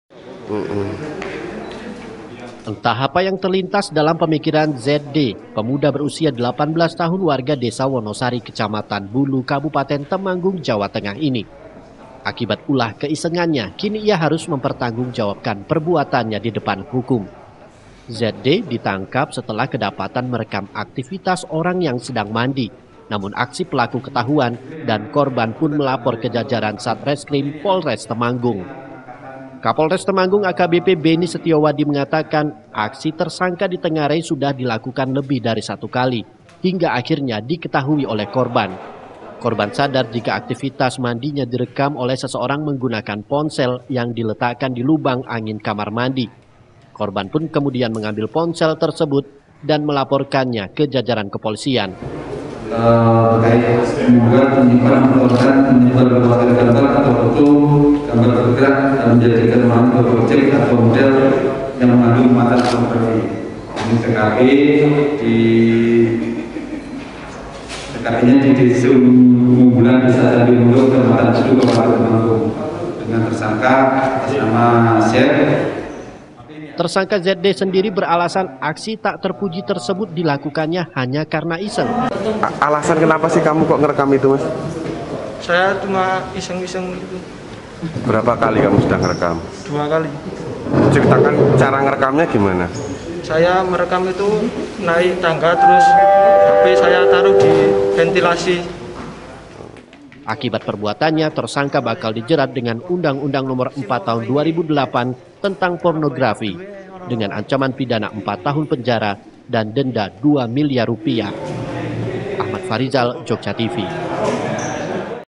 REKAM ORANG MANDI, PEMUDA DICIDUK POLISI !!!
JOGJA TV - Berdalih hanya iseng, seorang pemuda diciduk Aparat Polres Temanggung, karena aksinya merekam orang sedang mandi. Alhasil kini pelaku harus mendekam di sel tahanan dengan ancaman kasus pornografi.